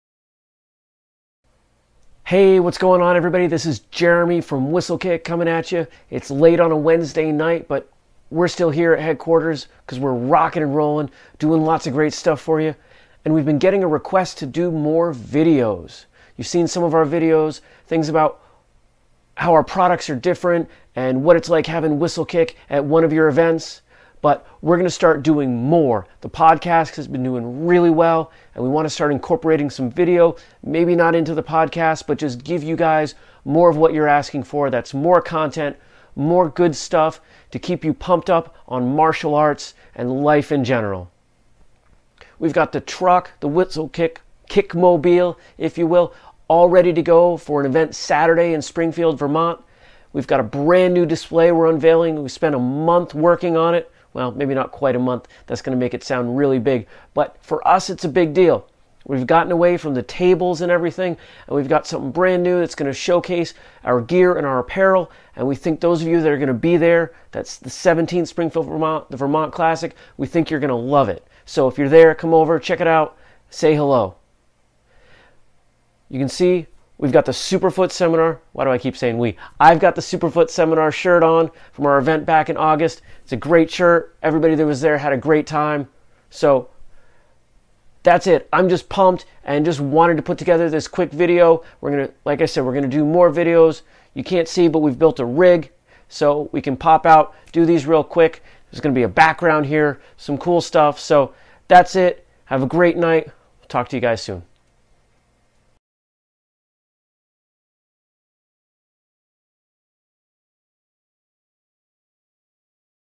Episode 1 - whistlekick Update Show
Our first episode - mostly a test - telling you what's going on at whistlekick.